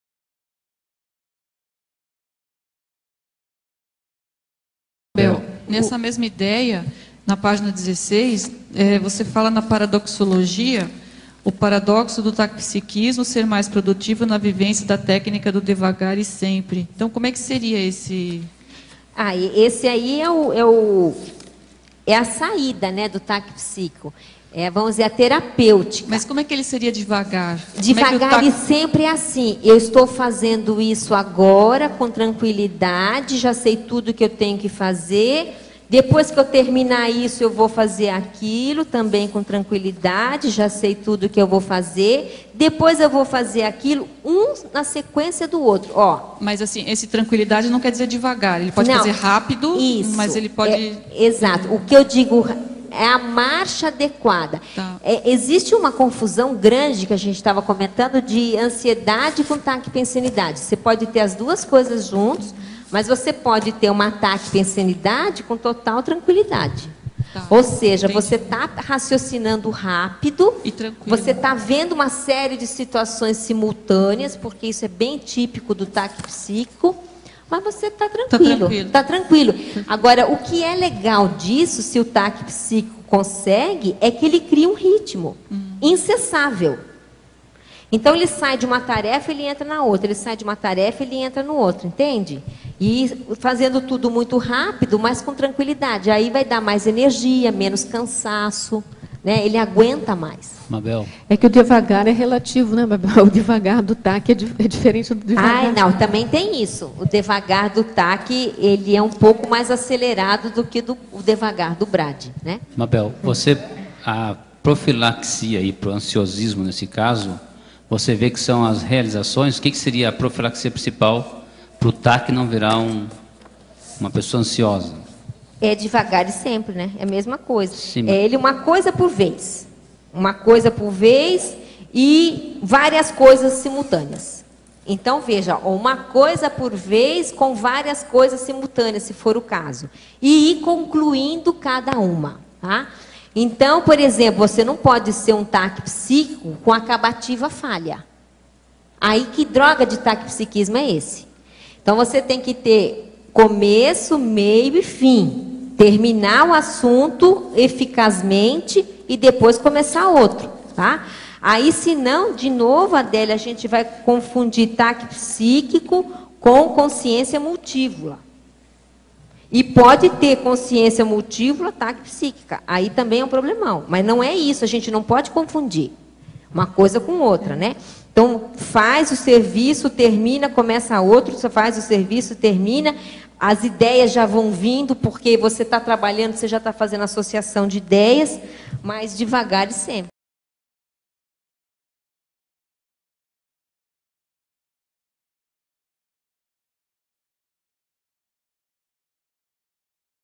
CONSCIENCIOLOGIA - Técnica do devagar e sempre - TAQUIPENSENIDADE HÍGIDA
Tertúlia - Aula 2295 - Qualificação do autotaquipsiquismo (Taquipensenologia) - Ao vivo todos os dias de forma gratuita nesse horário, 12:30 às 14:30. Clique lá e fique mais sábio (a). Tertúlias com o prof. Waldo Vieira